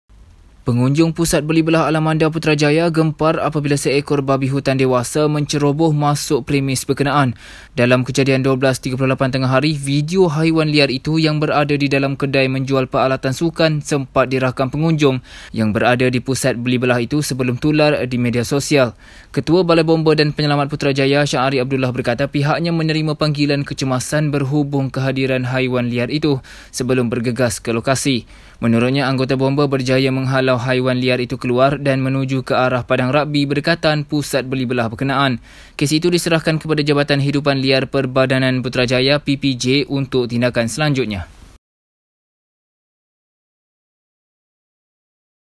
Babi hutan masuk pusat beli belah
Pengunjung pusat beli belah Alamanda Putrajaya , gempar apabila seekor babi hutan dewasa menceroboh masuk premis berkenaan.

Dalam kejadian 12.38 tengah hari tadi, video haiwan liar itu yang berada di dalam kedai menjual peralatan sukan sempat dirakam pengunjung yang berada di pusat beli itu sebelum tular di media sosial.